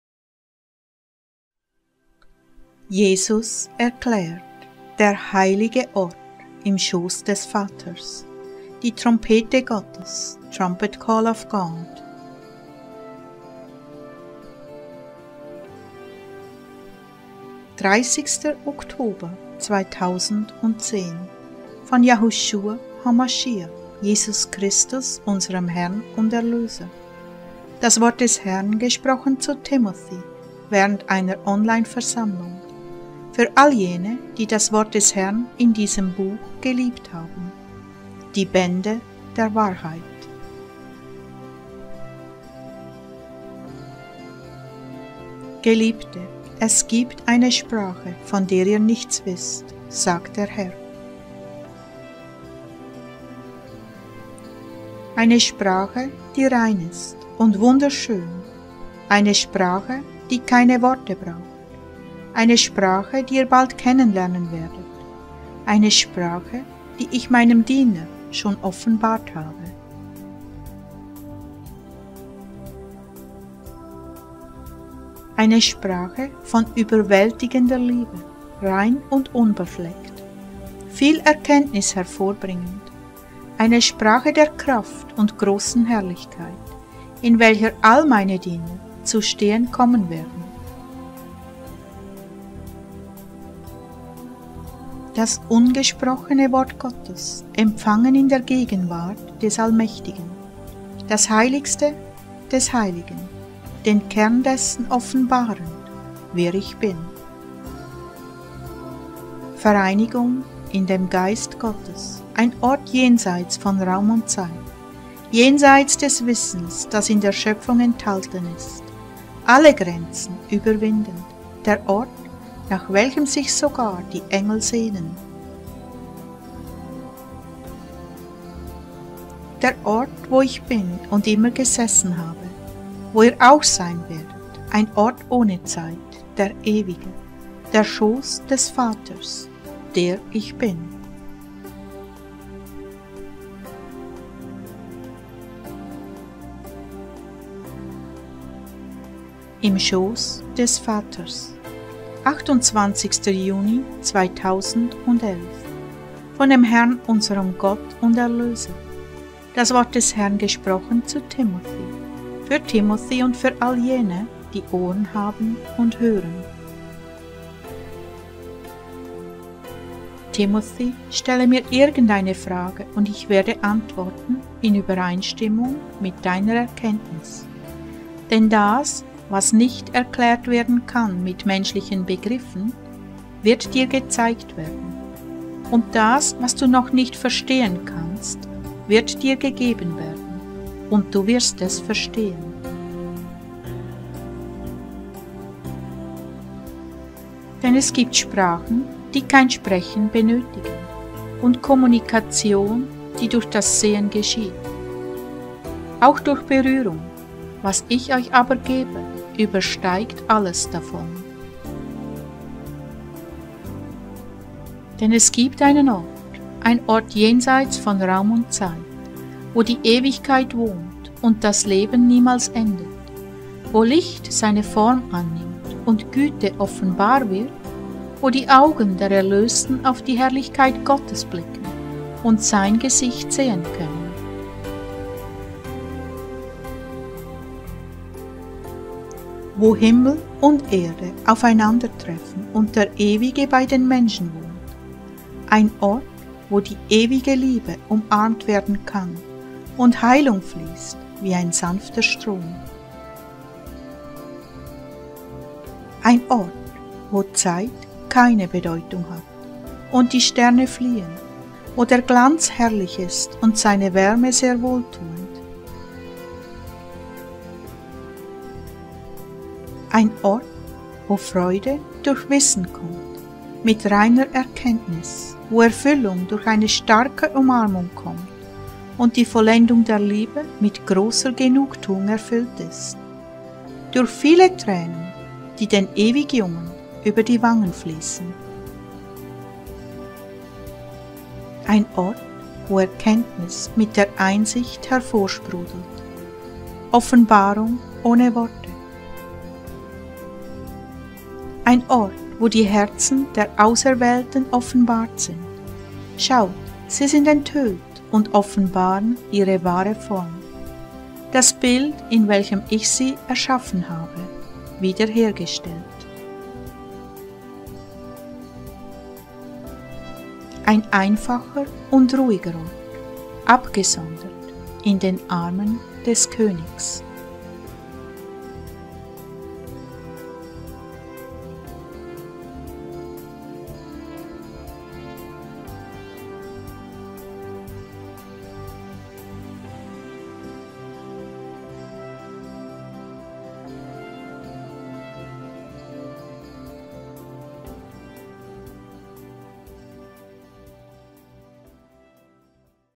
DER HEILIGE ORT IM SCHOSS DES VATER'S ❤️ DIE TROMPETE GOTTES
Jesus erklärt… DER HEILIGE ORT

30. Oktober 2010 – Von YahuShua HaMashiach, Jesus Christus, Unserem Herrn und Erlöser – Das Wort Des Herrn, gesprochen zu Timothy während einer Online Versammlung – Für all Jene, die das Wort des Herrn in diesem Buch geliebt haben… Die Bände der Wahrheit

Geliebte, es gibt eine Sprache, von der ihr nichts wisst, sagt Der Herr.

Eine Sprache, die rein ist und wunderschön, eine Sprache, die keine Worte braucht, eine Sprache, die ihr bald kennenlernen werdet, eine Sprache, die Ich Meinem Diener schon offenbart habe…

Eine Sprache von überwältigender Liebe, rein und unbefleckt, viel Erkenntnis hervorbringend. Eine Sprache der Kraft und grossen Herrlichkeit, in welcher all Meine Diener zu stehen kommen werden…

Das ungesprochene Wort Gottes, empfangen in der Gegenwart Des Allmächtigen, Das Heiligste des Heiligen, den Kern dessen offenbarend, wer Ich bin…

Vereinigung in Dem Geist Gottes… Ein Ort jenseits von Raum und Zeit, jenseits des Wissens, das in der Schöpfung enthalten ist, alle Grenzen überwindend, der Ort, nach welchem sich sogar die Engel sehnen…

Der Ort, wo Ich bin und immer gesessen habe, wo ihr auch sein werdet… Ein Ort ohne Zeit, Die Ewigkeit… Der Schoss Des Vater’s… Der ICH BIN.

Übersicht der letzten 20 Botschaften !

PERSÖNLICHES WORT VON DEM HERRN!